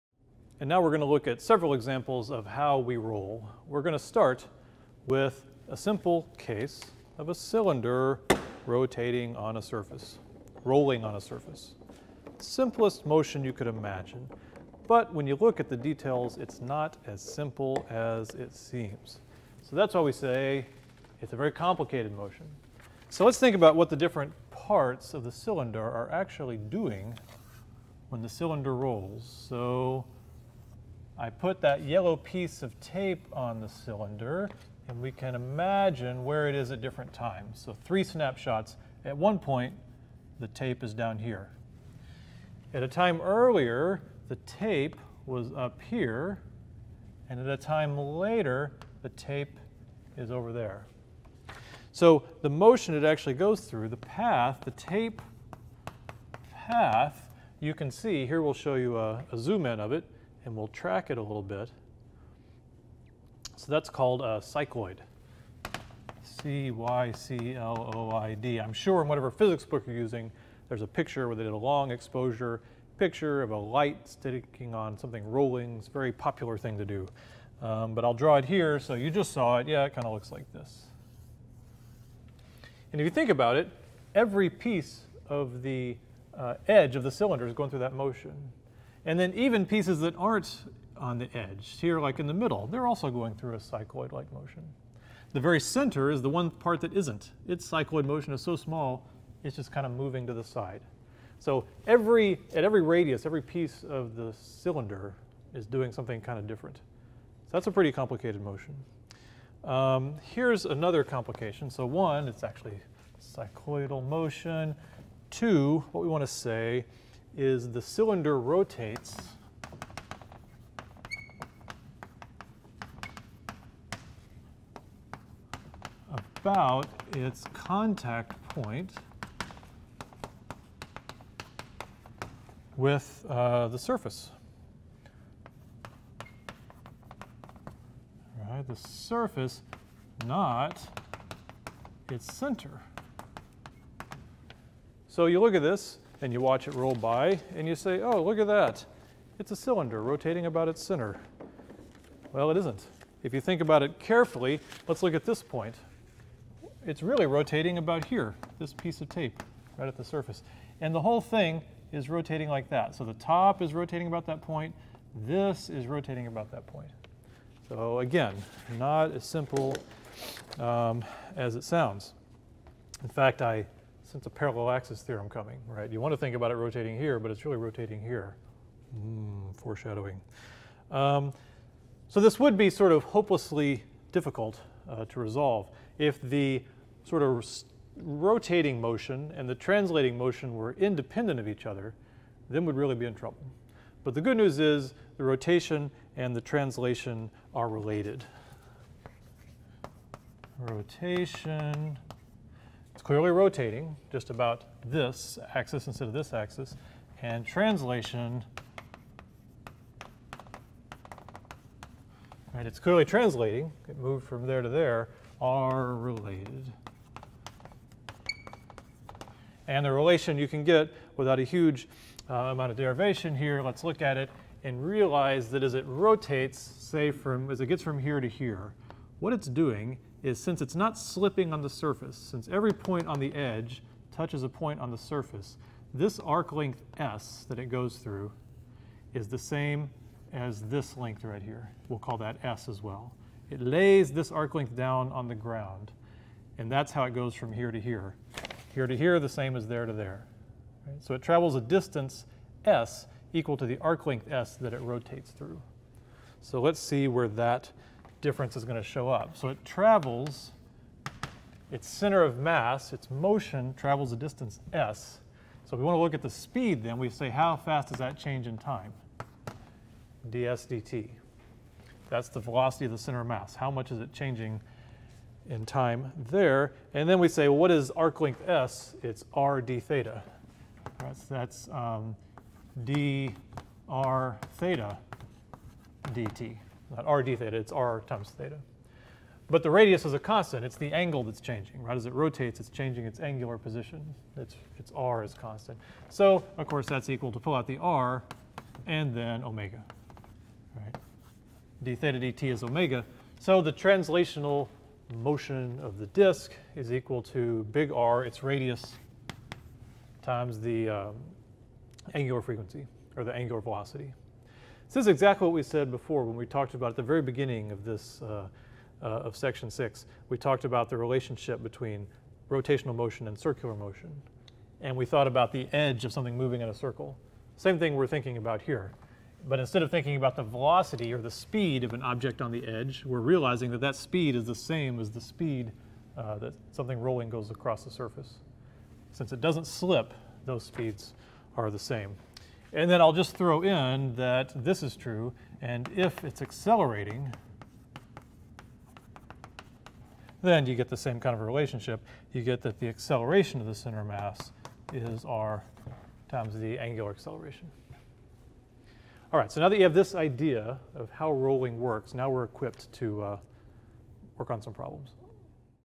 PHYS 101 | How We Roll 1 - Rolling Cylinder
The concept and definition of torque, with three examples that we have seen.